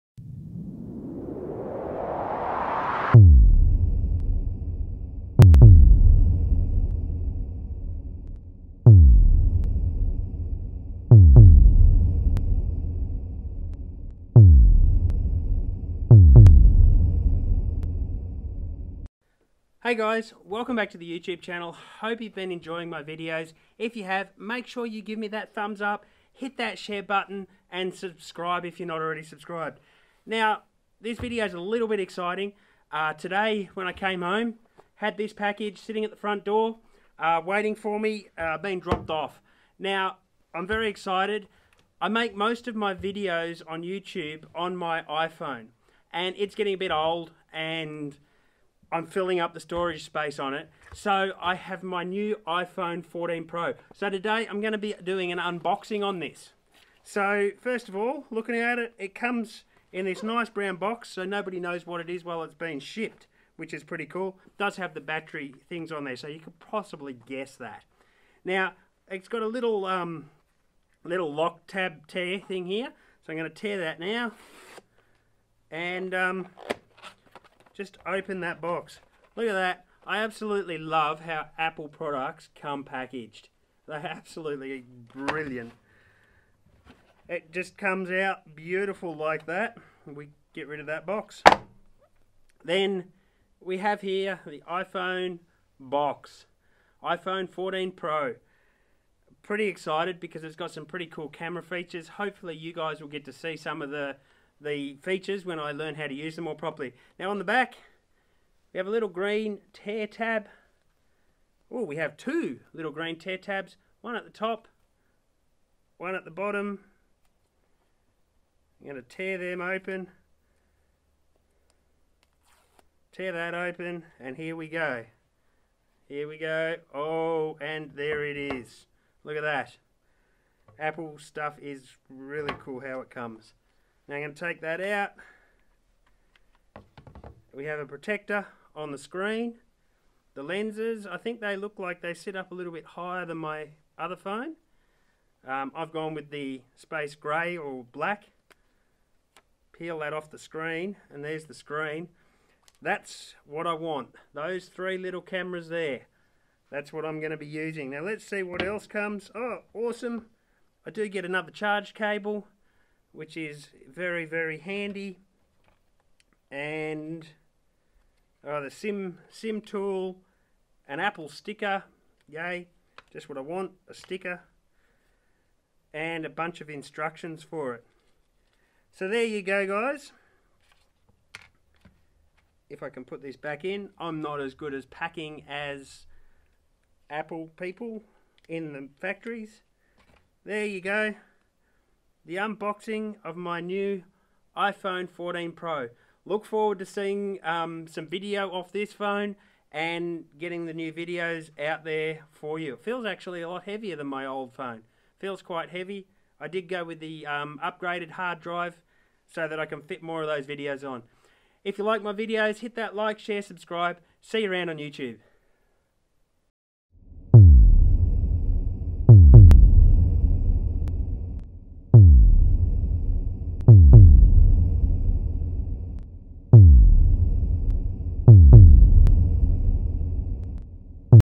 iphone 14 pro unboxing
I finally received my new iPhone 14 pro.
decided to open it on camera.